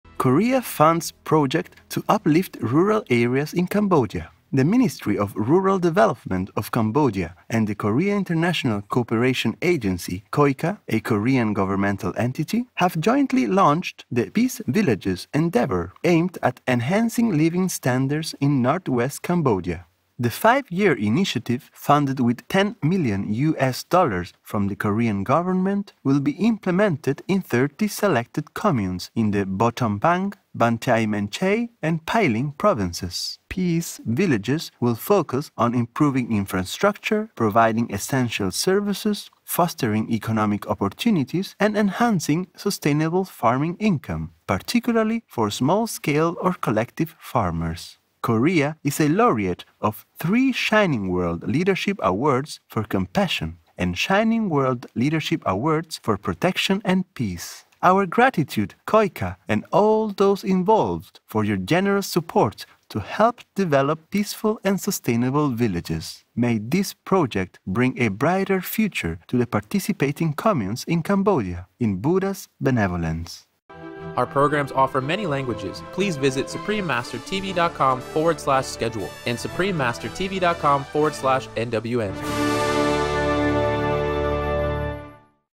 韓國支援柬埔寨提高西北農村省分的生活水準
Korea supports Cambodia in work to bolster living standards in rural northwest provinces